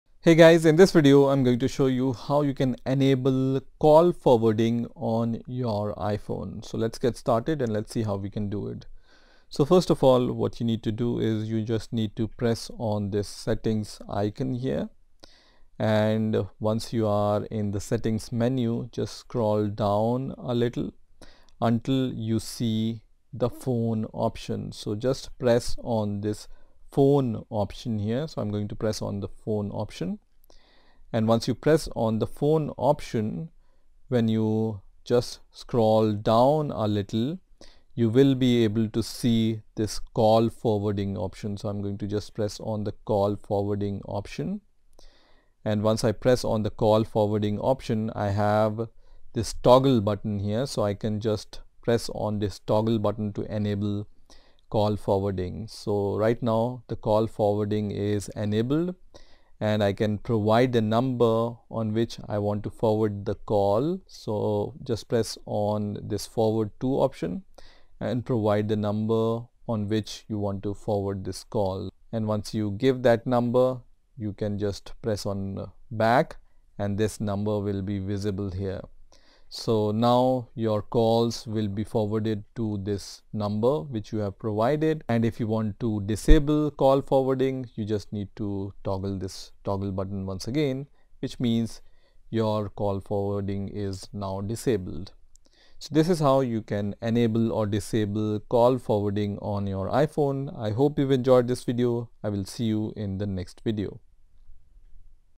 How to Enable and Disable Call Forwarding in iPhone
In this video we will see How to Turn Off Call Forwarding on an iPhone, iPad, or iPod touch. So let us see How to Set up or Disable Call Forwarding in iOS for iPhone.